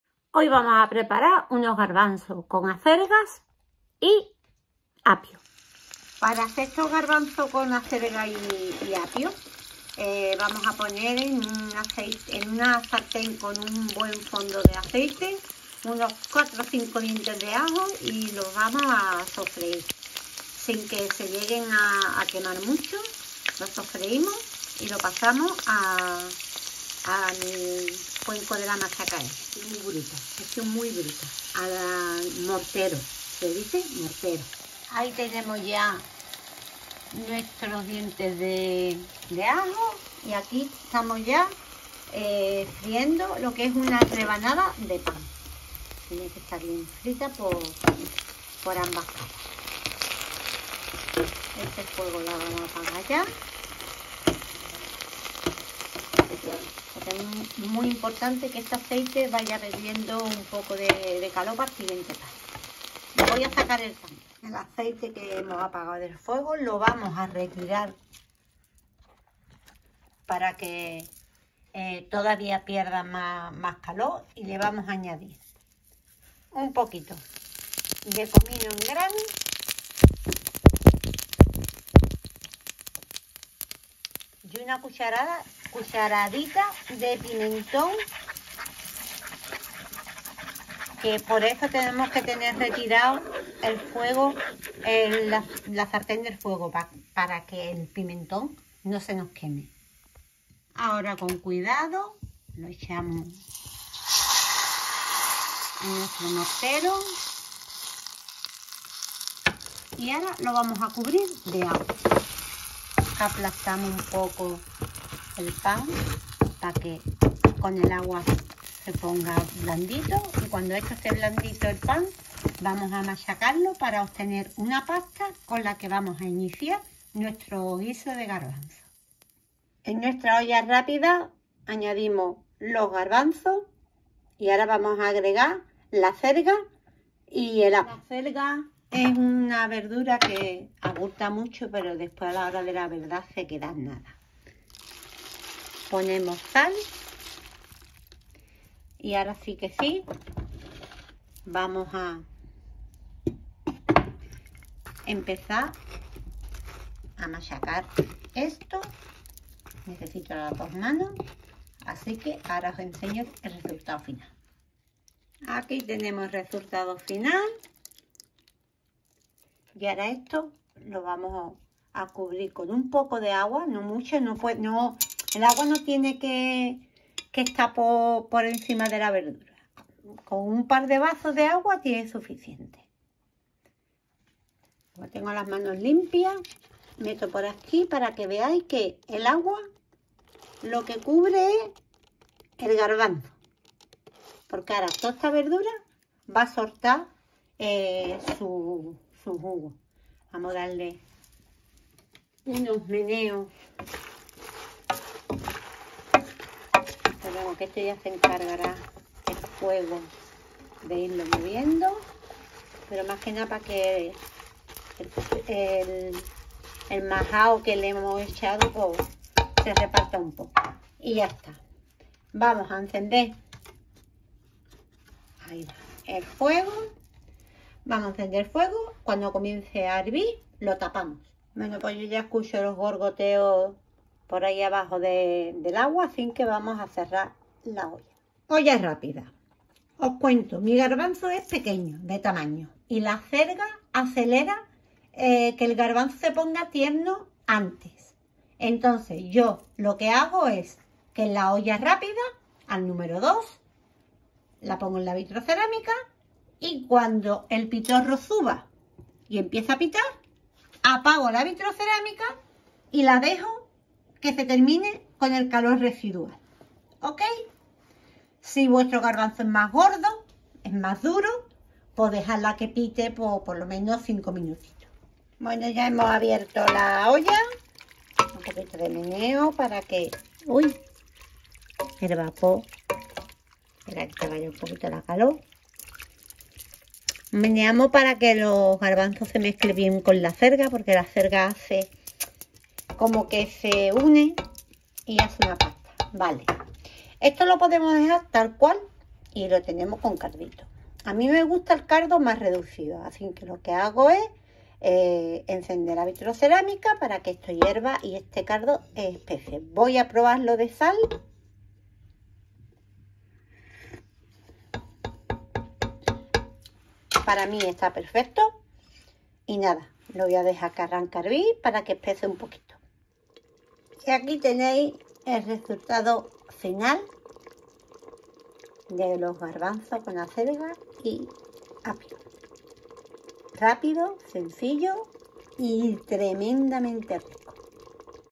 Garbanzos con acelgas y apio
Sencillo y rápido guiso de Garbanzos con acelgas y apio